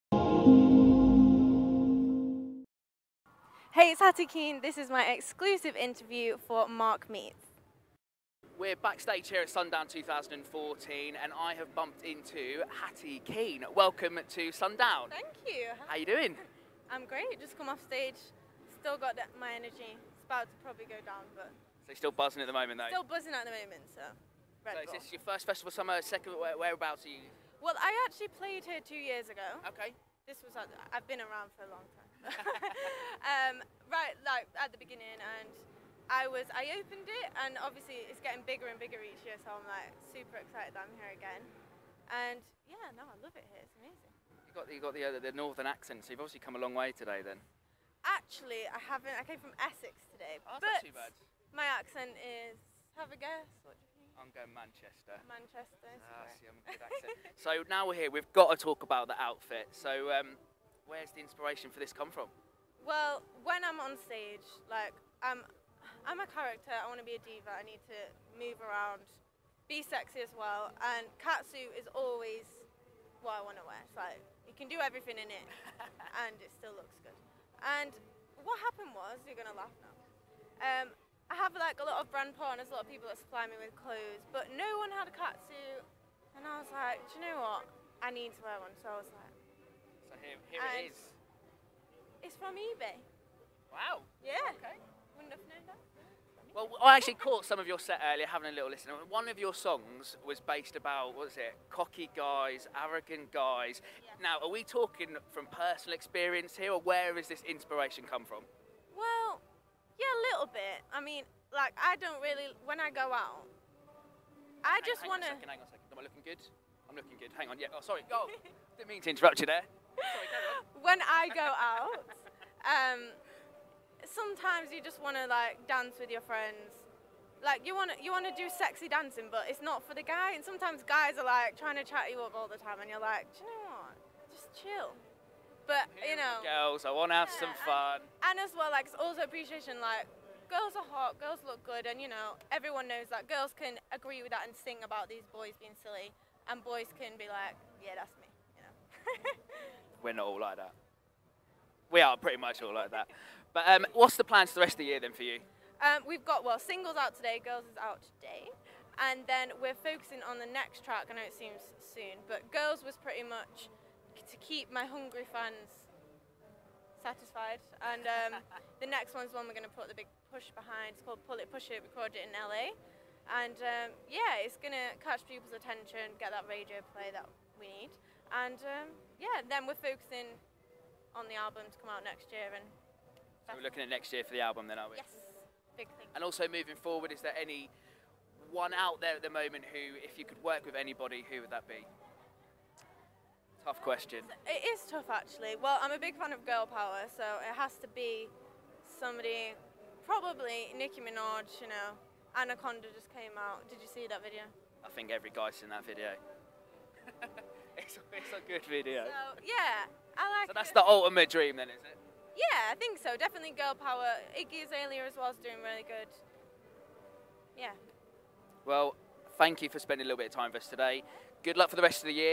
Hatty Keane Interview | MarkMeets 2014
Hatty Interview | MarkMeets 2014. Interview by Adam Oakley. Camera  and edit by Mark Boardman.